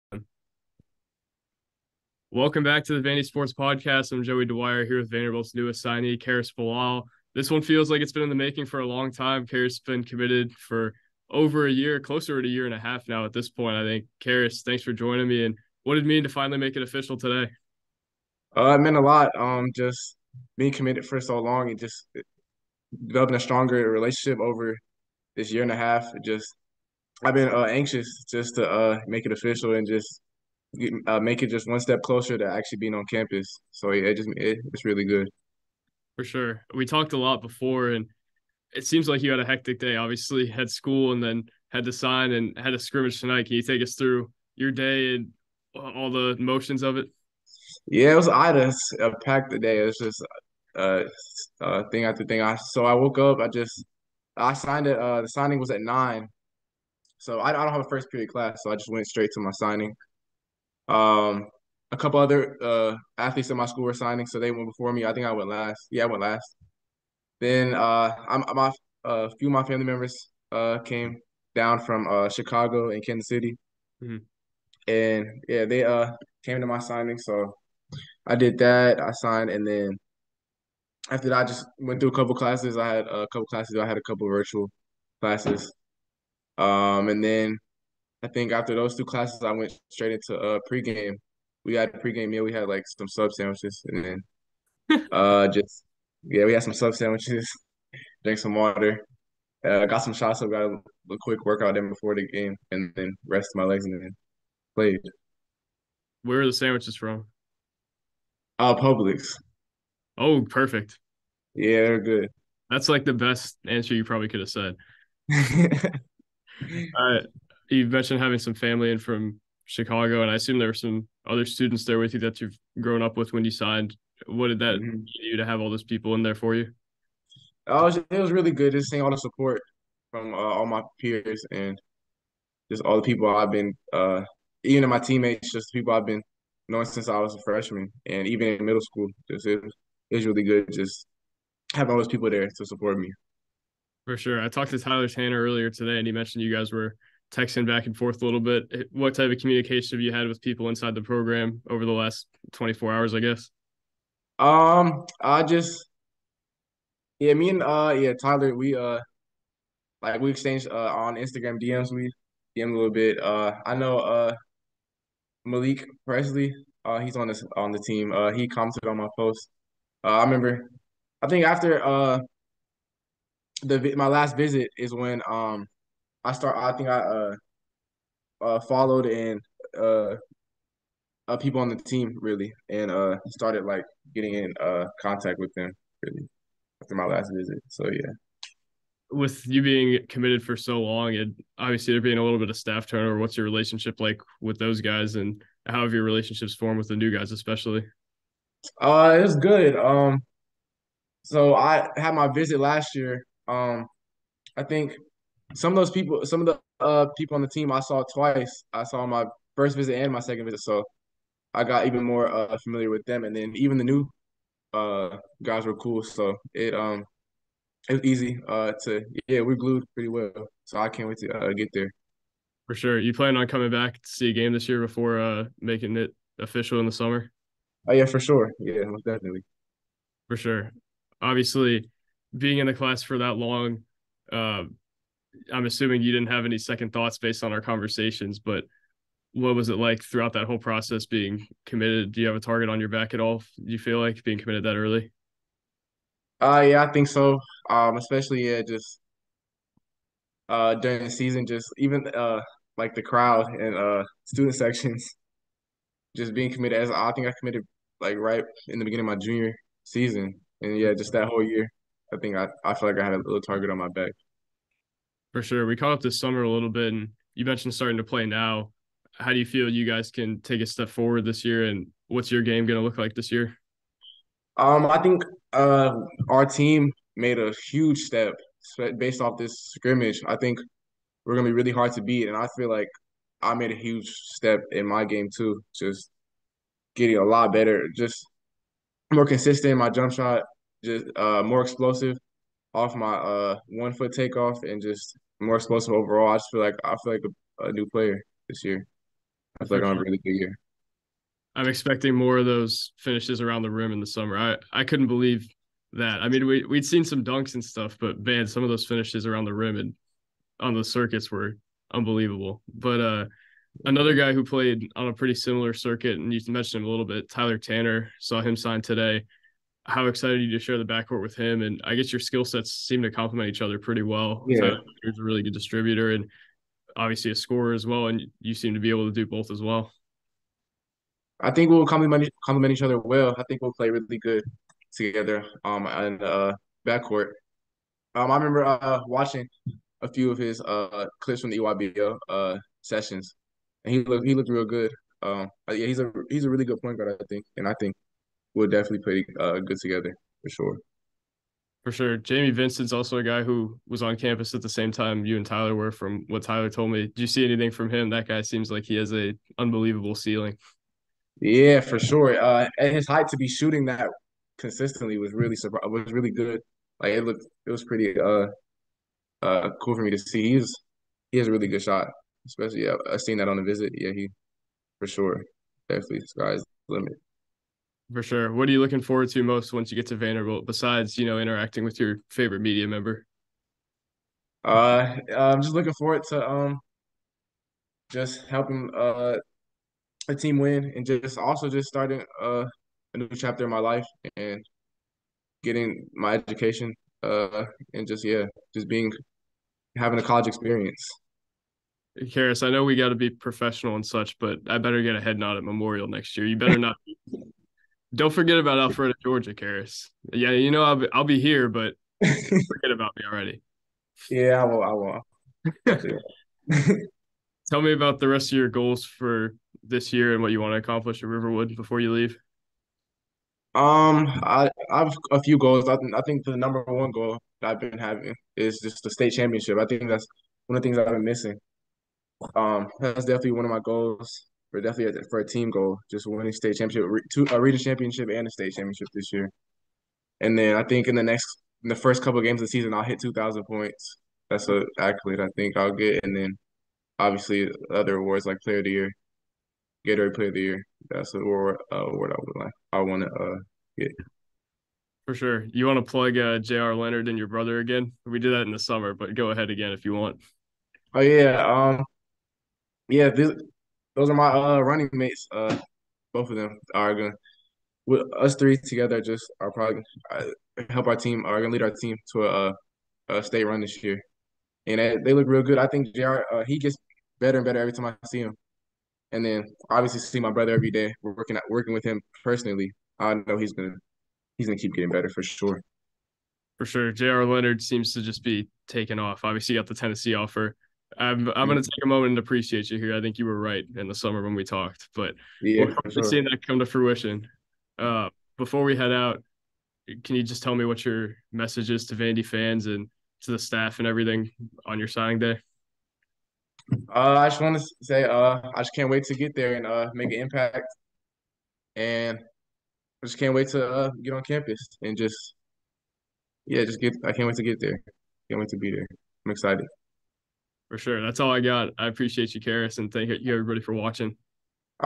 Karris Bilal reflects on signing with Vanderbilt
This is the YouTube channel for VandySports.com, an independent website founded in 2003 that covers Vanderbilt football, baseball and men's basketball.